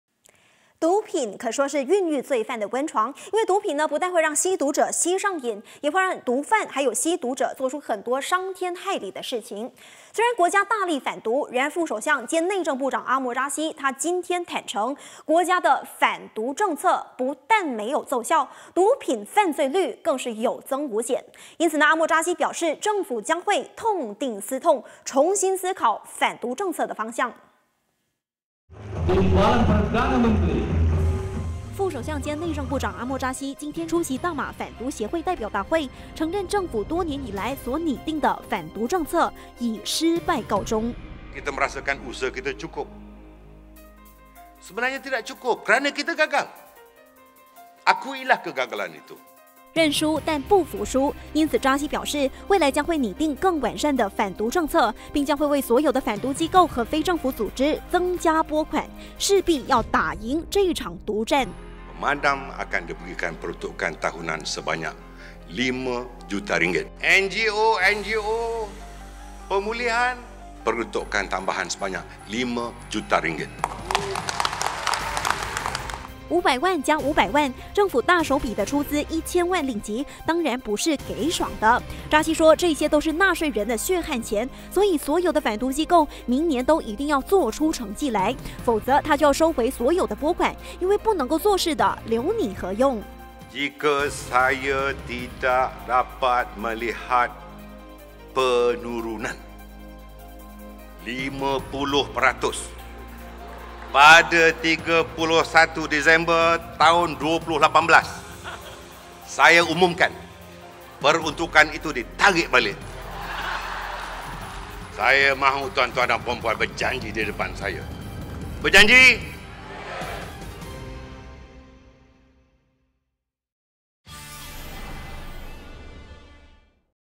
Prime Talk 八点最热报 17/12/17 - 扎希: 政府毒战打败仗!
副首相兼内政部长阿末扎希今天表示，毒品犯罪率有增无减，因此政府将会痛定思痛，再拨1千万令吉给个贩毒机构，势必要打赢毒战。